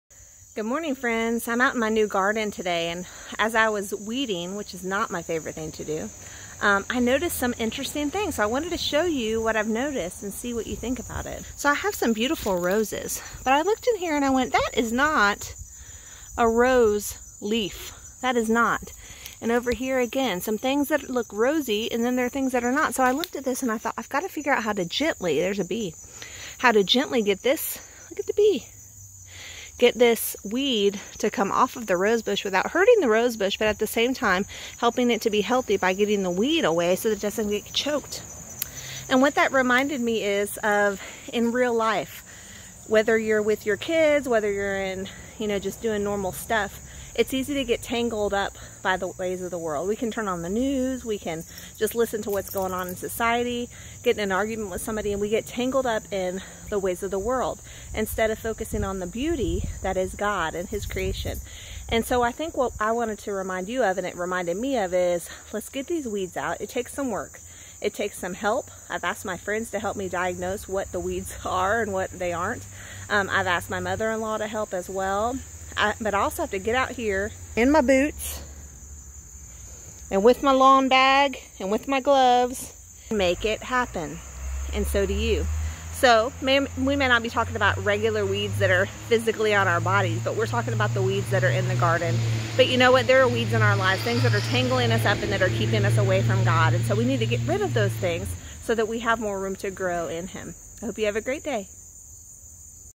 Don't Get Tangled Up
Here's a short video describing how weeds (and things of the world) can take our focus off of what matters and how to get untangled from your weeds.